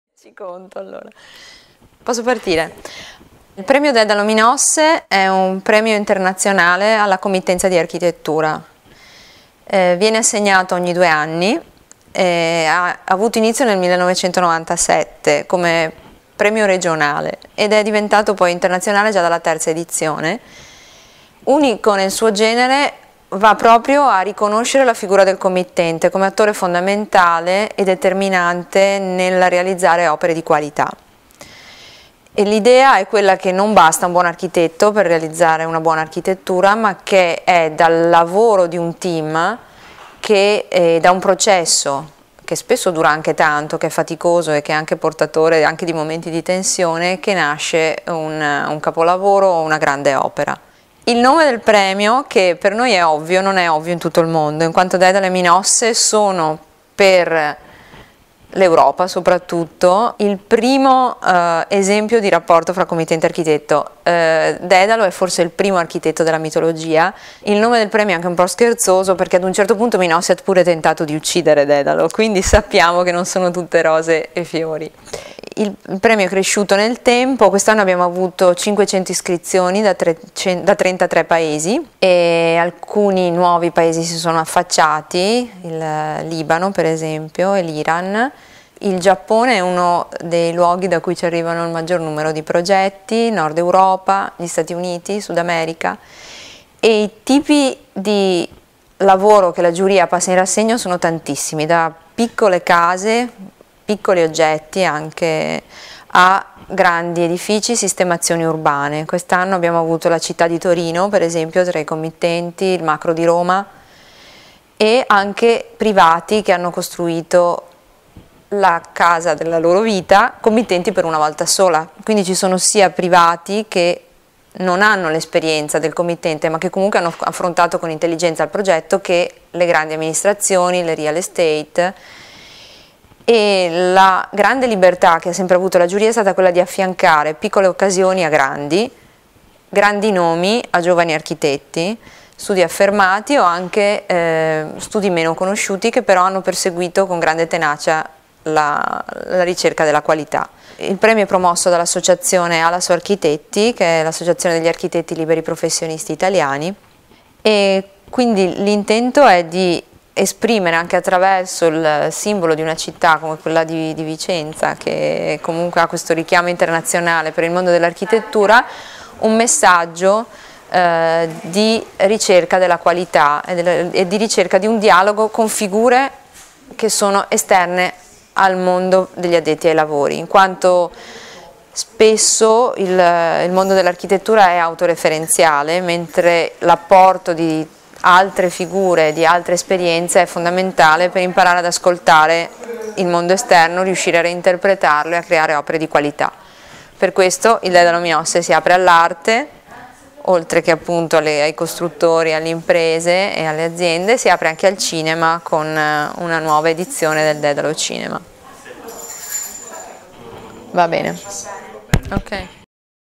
Amor Vacui / Interviews. PREMIO DEDALO MINOSSE 2013/14. MARCELLA GABBIANI
PREMIO DEDALO MINOSSE 2013/14. PREMIO INTERNAZIONALE ALLA COMMITTENZA DI ARCHITETTURA. (INTERNATIONAL PRIZE FOR COMMISSIONING A BUILDING)

NONA EDIZIONE 2013/2014

CONTRIBUTO (CONTRIBUTION): MARCELLA GABBIANI
(Direttrice del Premio Dedalo Minosse)

AMOR VACUI / INTERVIEWS
Interviews and video editing: MARZIO DI PACE, ROSA SESSA

Follow AV on FB: Amor Vacui / Pretesti di Architettura